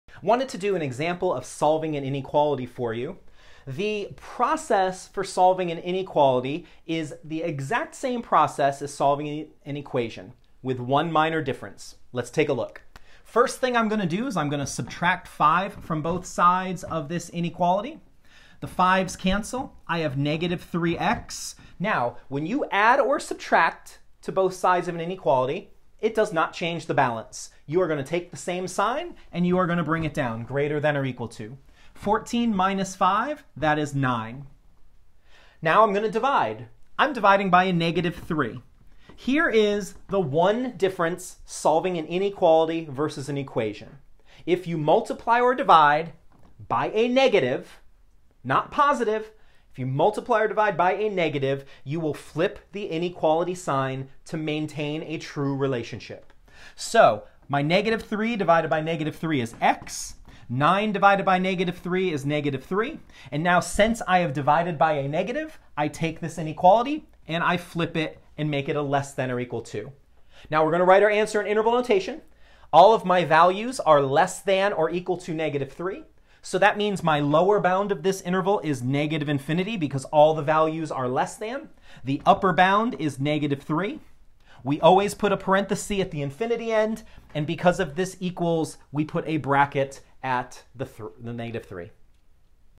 Solving a Linear Inequality
An example of solving a linear inequality to demonstrate when to flip the sign.